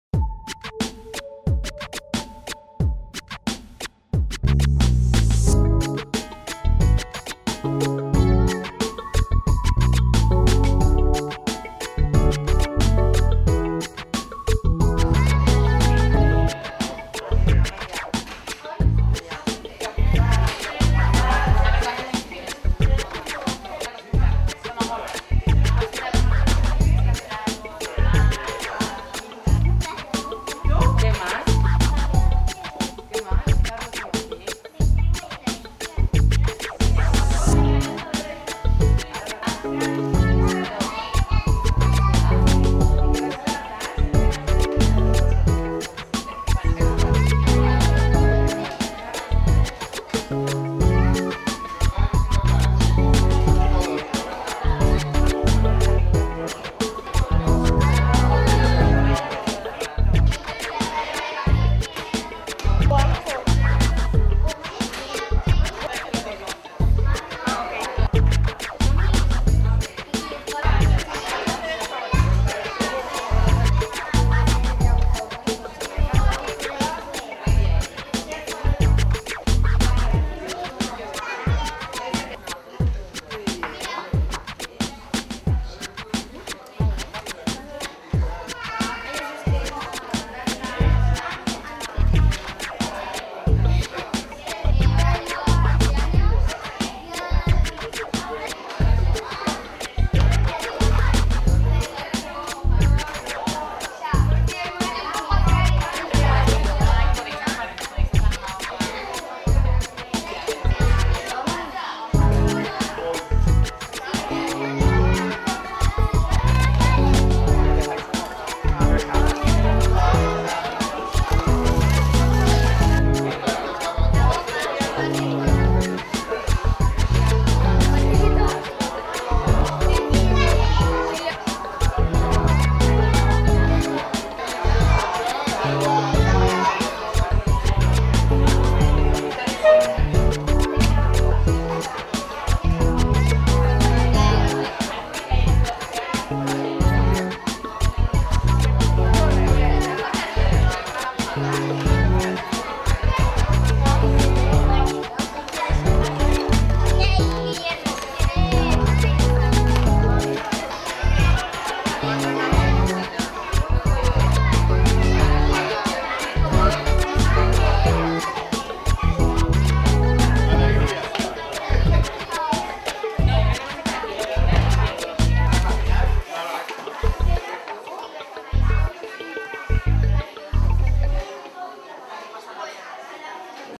Concurso de Pintura Infantil 2013
Concurso de Pintura Infantil organizado por el Club de Vela Puerto de Andratx y que se celebro el pasado 3 de Agosto de 2013 en el local social del Club.